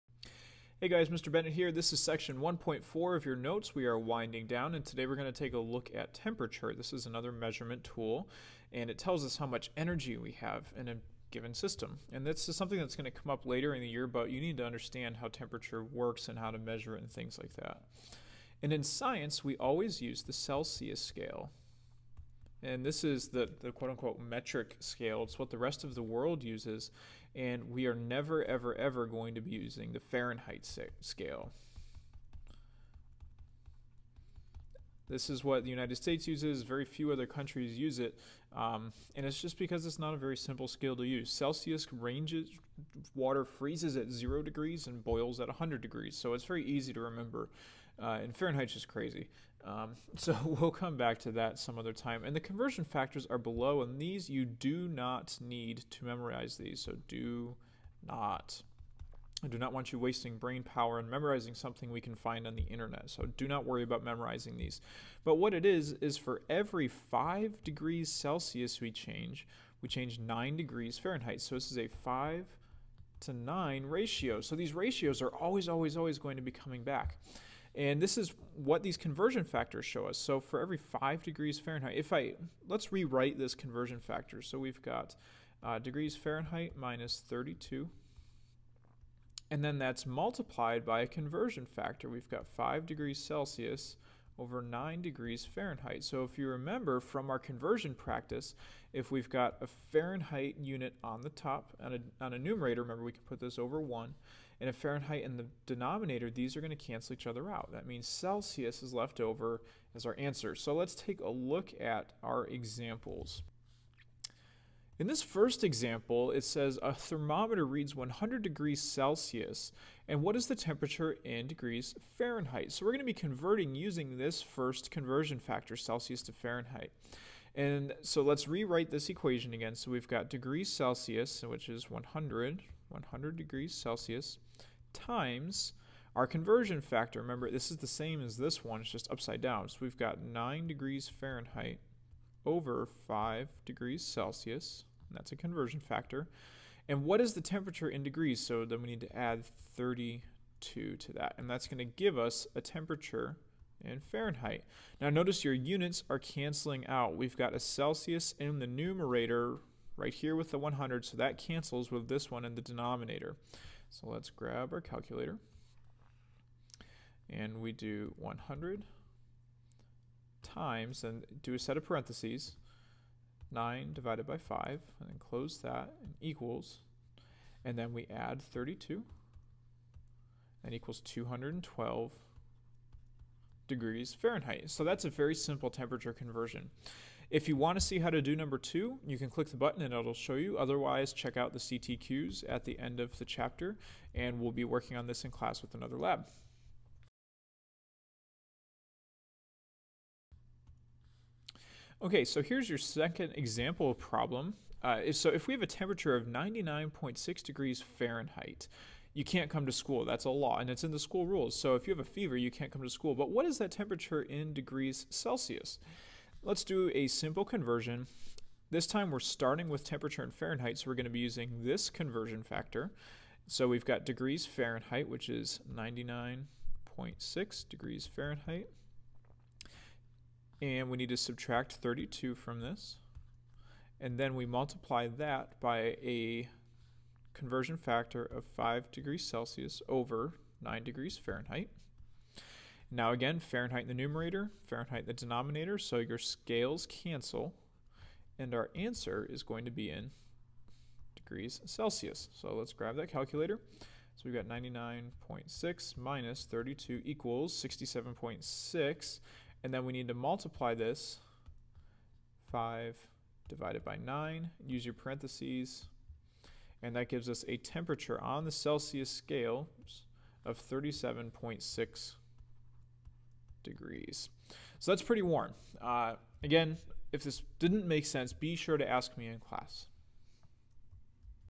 Temperature Conversions
Another American confusion: measuring temperature in Celsius (or Centigrade). The Fahrenheit scale is not used in science, but good news: Celsius is much easier, just like metric. This video shows how to do simple conversions between the two scales.